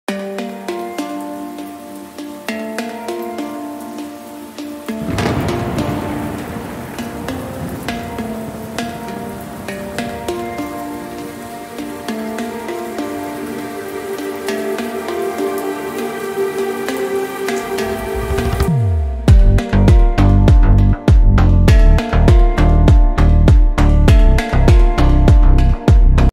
3.5 Marla house for Sale in Wakeel Colony, Rawalpindi
3.5 Marla house for Sale in Wakeel Colony, Rawalpindi

 Bathrooms: 3
 Bedrooms: 3
 Stories: 2
 Demand: Rs.10,000,000
 Electricity: Available
 Water: Water Tanker
 Gas: Available in Street

 Location: Wakeel Colony,Rawalpindi

 Inventory ID = 2265